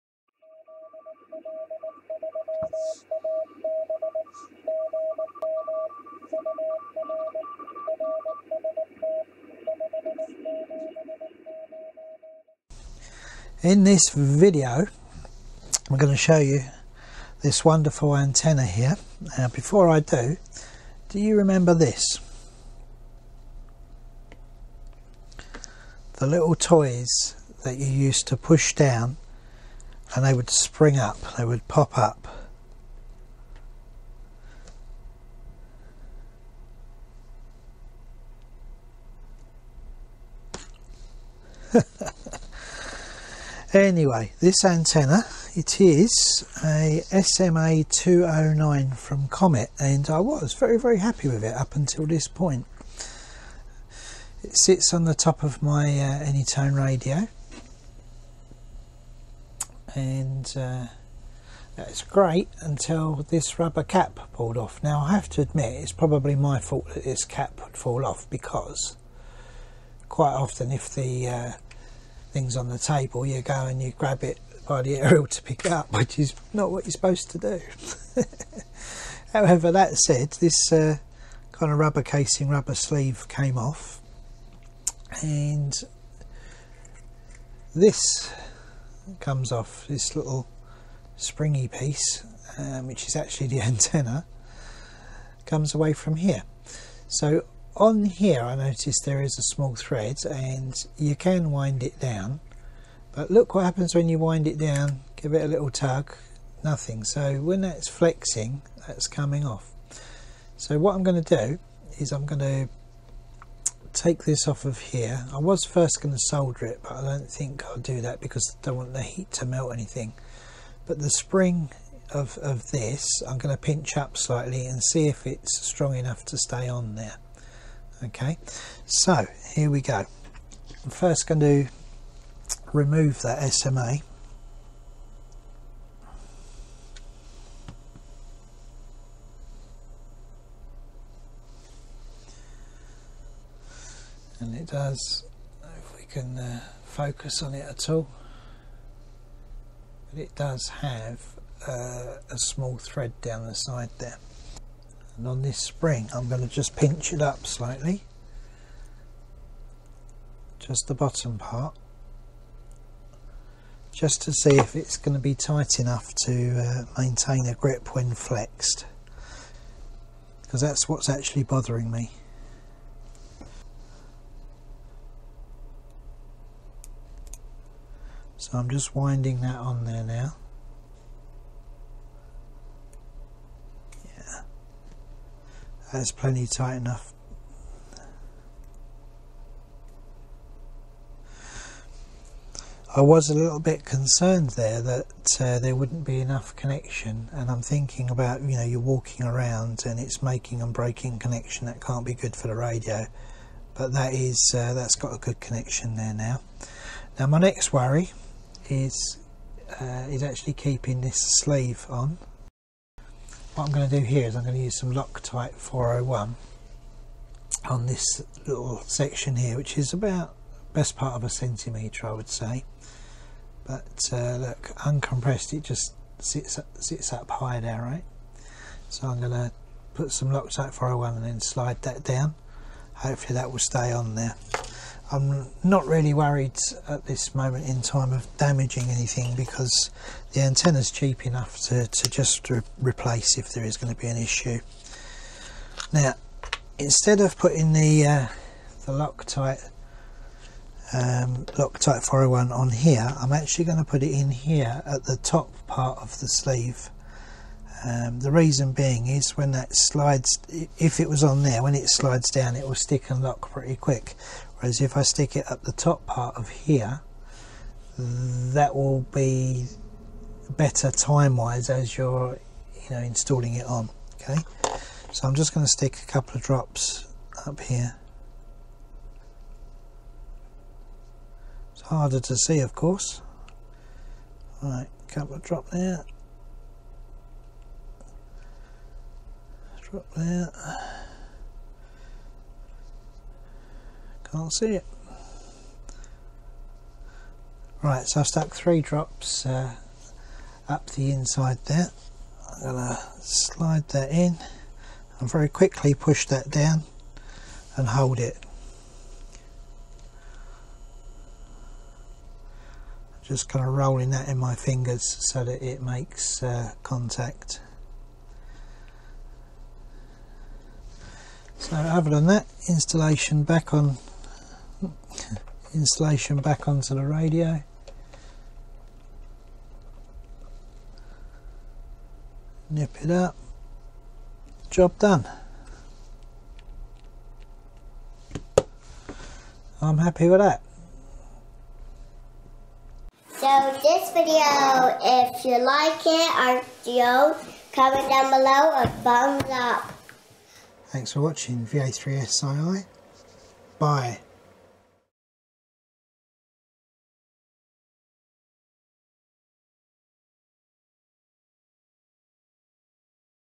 Repairing Comet SMA209
Repair of a wonky Comet SMA209 which seperated. Overall a good little antenna. The antenna spring needed pinching up so it gripped the base better. then the rubber enclosure needed adhesive to make it stay in the correct position.